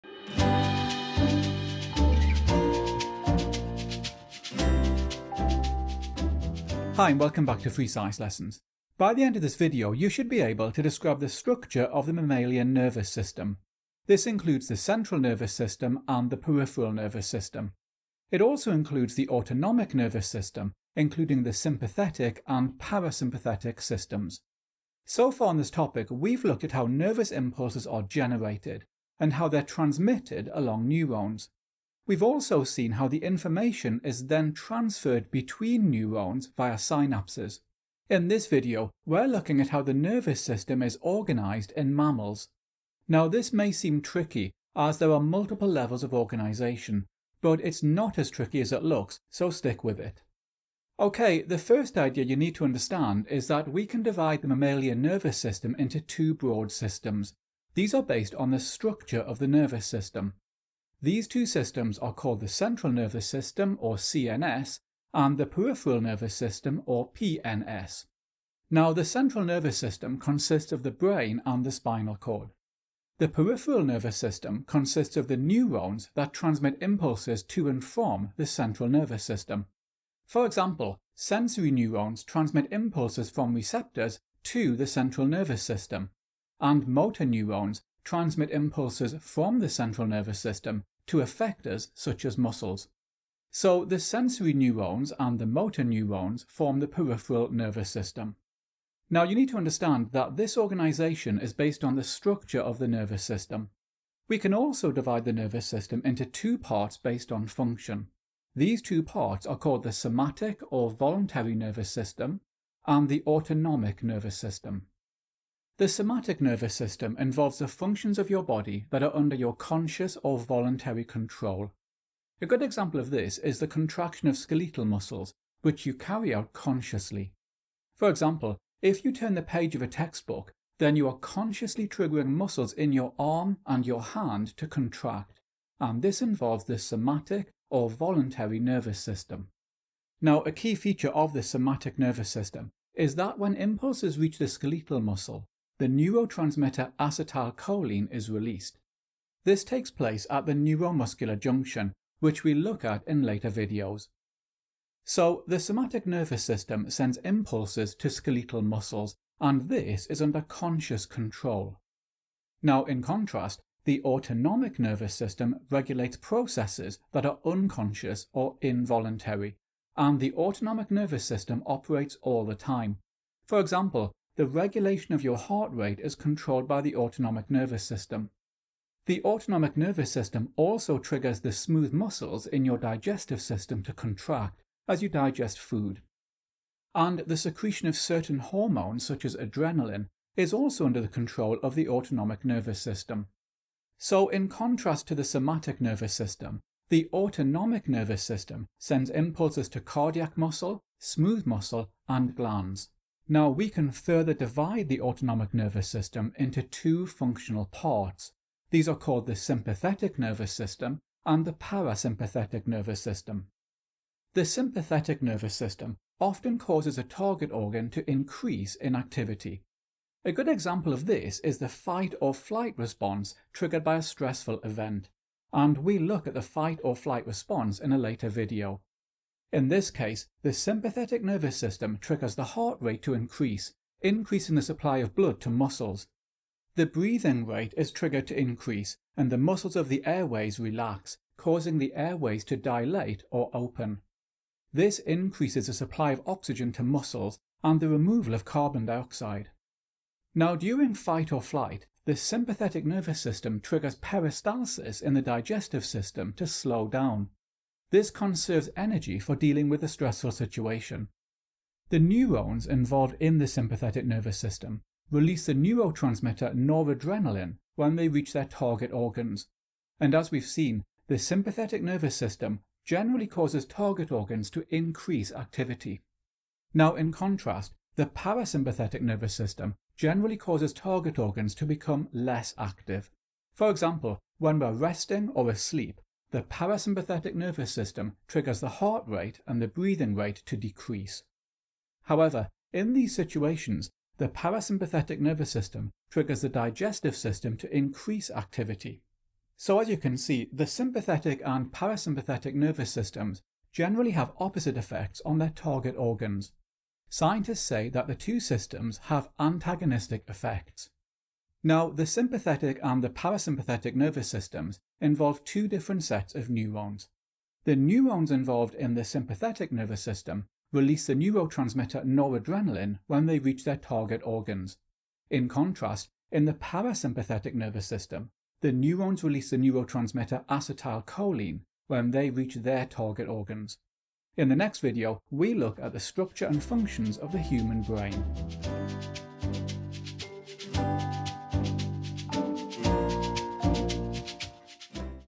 A Level Biology Revision (Year 13) "Organisation of the Nervous System"
In this video, we look at the organisation of the mammalian nervous system. We explore the central nervous system and the peripheral nervous system and then the somatic (voluntary) and autonomic nervous systems. Finally, we look at the functions of the sympathetic and parasympathetic nervous systems.